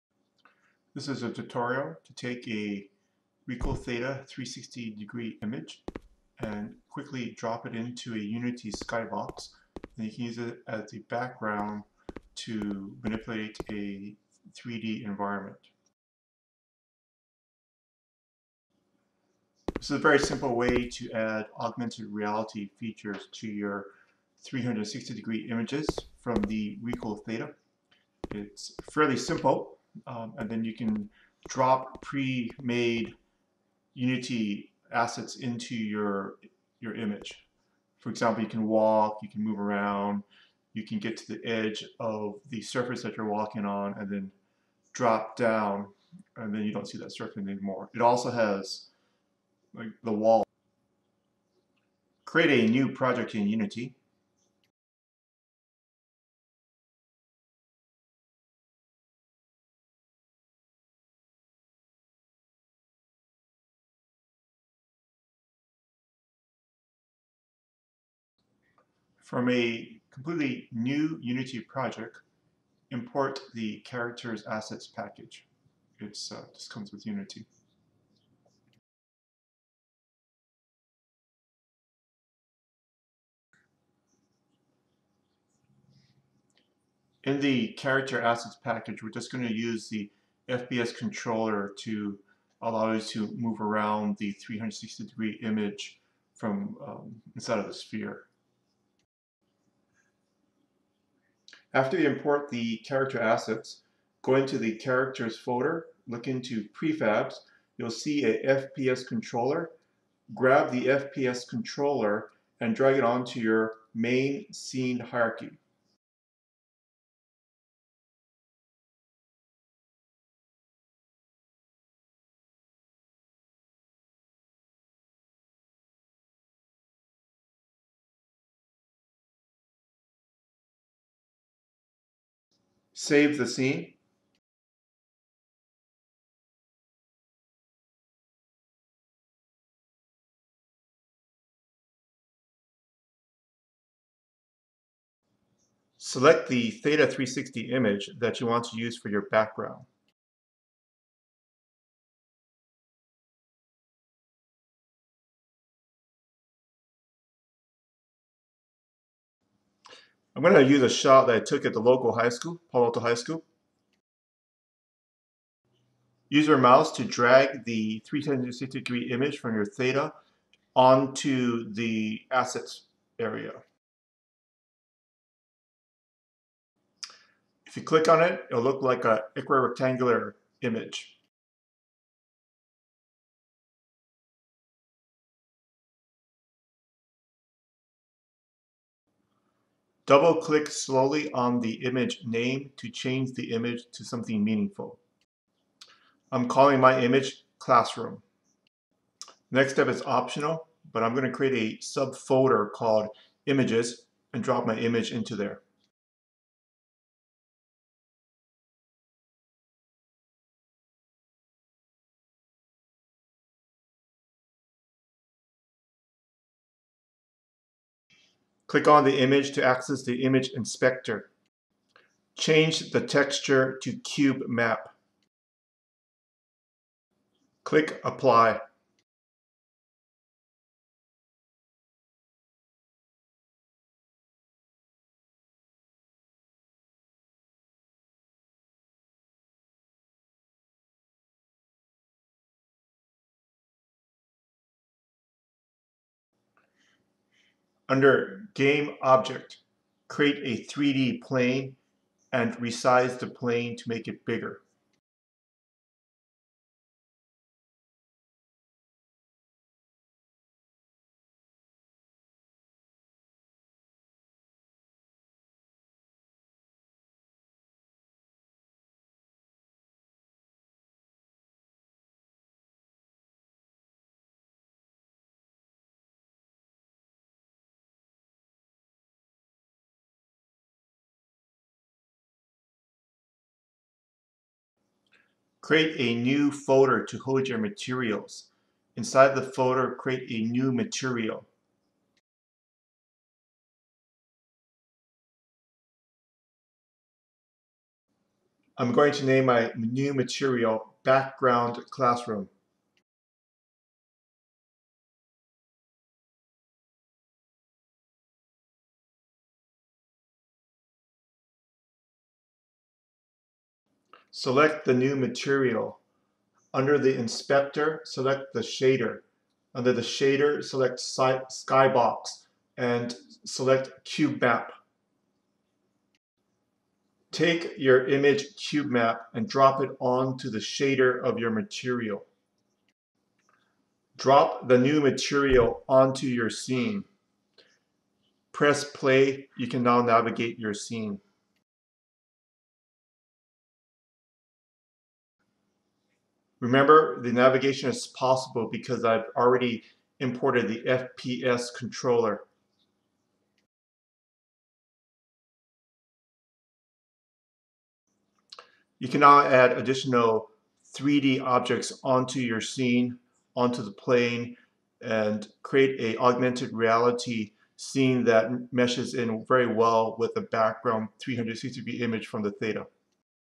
Beginner Tutorial: Use 360 RICOH THETA Images in Unity for Augmented Reality
Beginner tutorial to use RICOH THETA 360 images in Unity with Skybox cubemap. Walk around a plane with augmented reality objects, using the 360 image as the background for your scene. Super simple way to start your next augmented reality project.